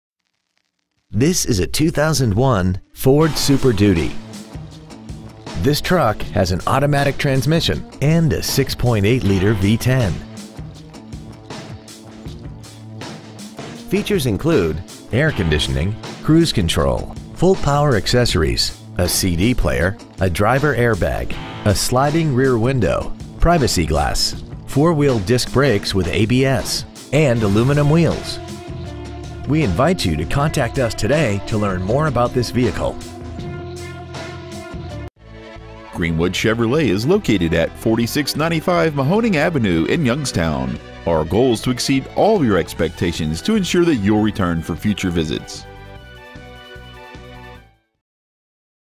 Pre-Owned 2001 Ford Super Duty Youngstown OH
We have been honored to serve the Youngstown OH area , we promise that your experience at our dealership will exceed your expectations ! Year : 2001 Make : Ford Model : Super Duty Engine : 6.8L 10-Cylinder Trans . : automatic Exterior : Red Miles : 75,646 Interior : Medium Graphite Stock : E35956B Greenwood Chevrolet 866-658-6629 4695 Mahoning Ave. Youngstown , OH 44515